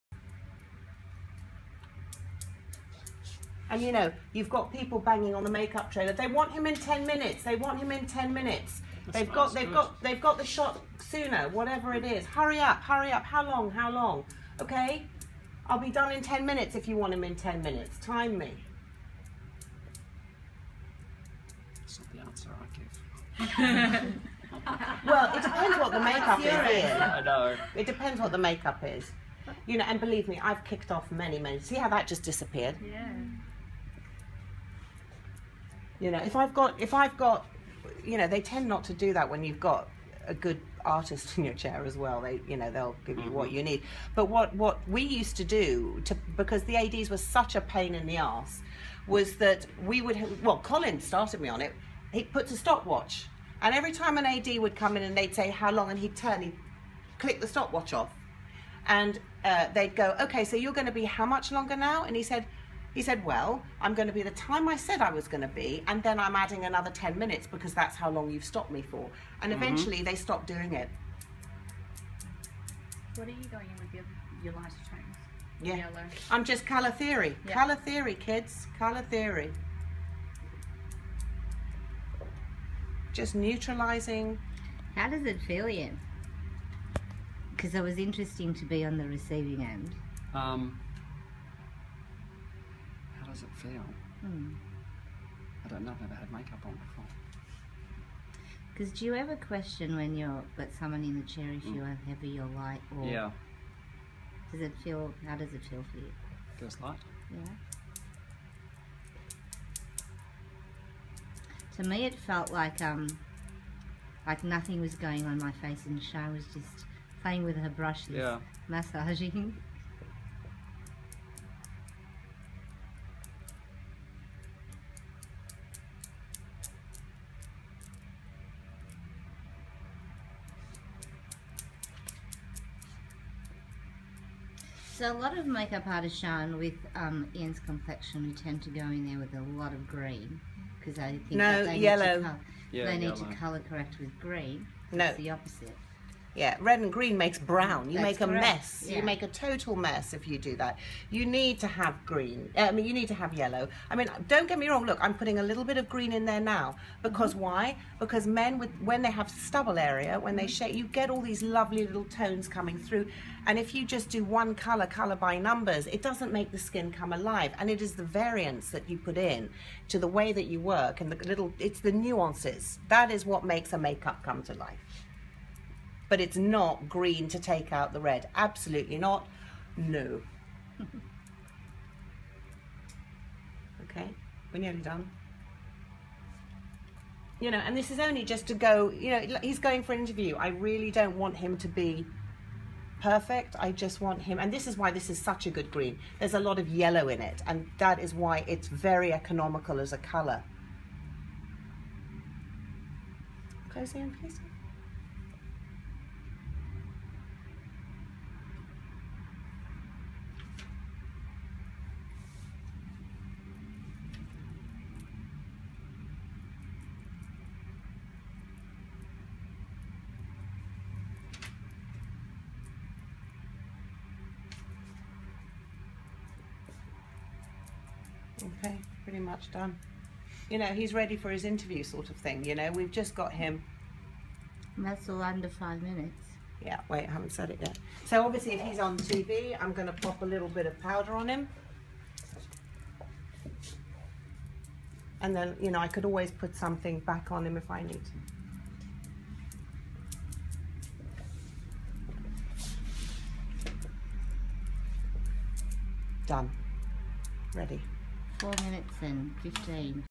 Sian's guide to simple men's makeup for tv or camera in under five minutes
Sian demonstrates quick natural makeup for tv character or movie makeup. Colour correction and evening out without it looking like makeup. Ready and finished in under 5 minutes with setlife stories discussed with all the attendees..